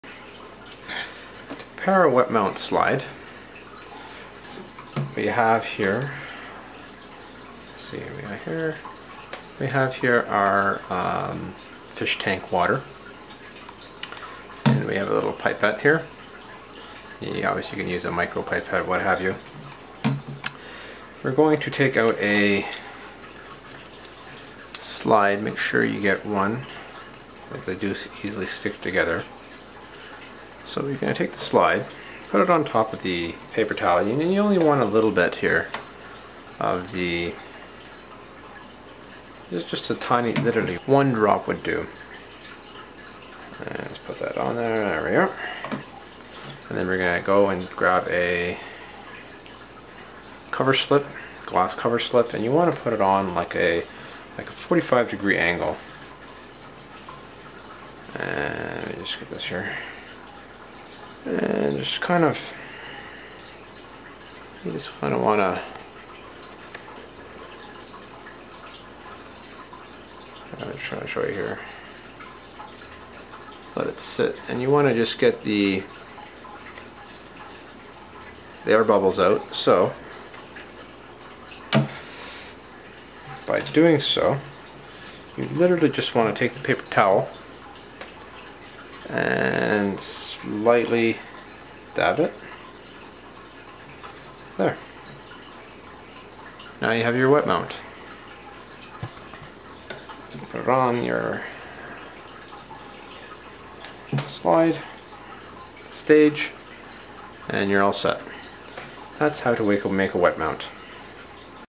How to prepare a wet mount slide for microscope viewing
Preparing a wet mount to view aquarium water under a microscope.